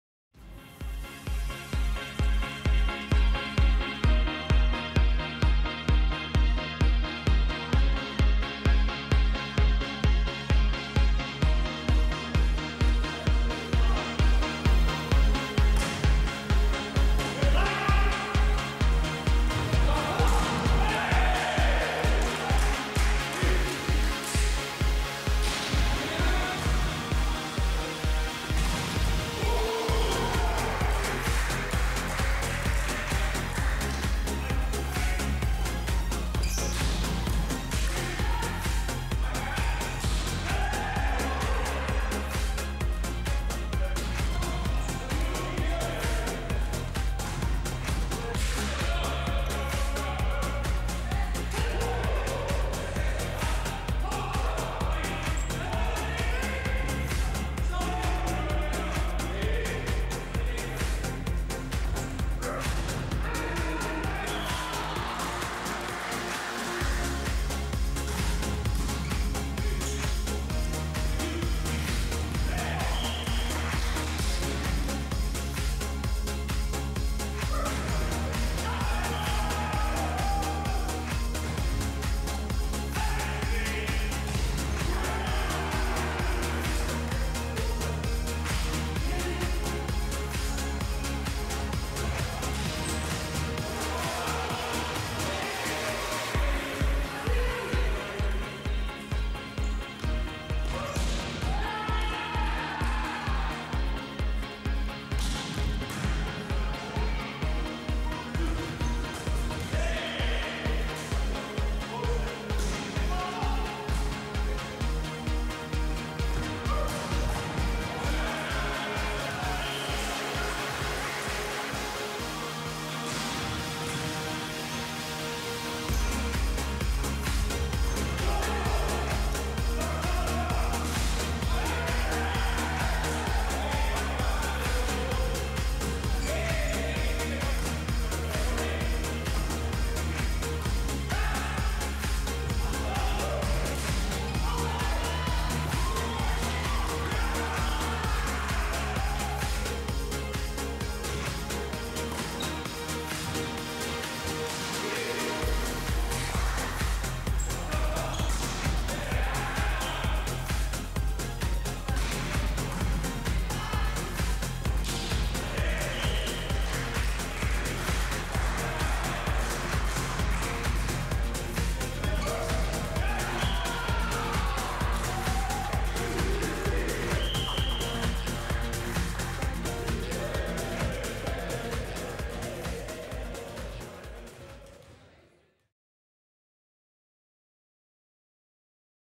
City of Edinburgh Men vs Bon Accord
Men's Volleyball Highlights from Mearns Academy where City of Edinburgh overcame Bon Accord (Aberdeen) 3-0 away from home for their to go 5&1 for the season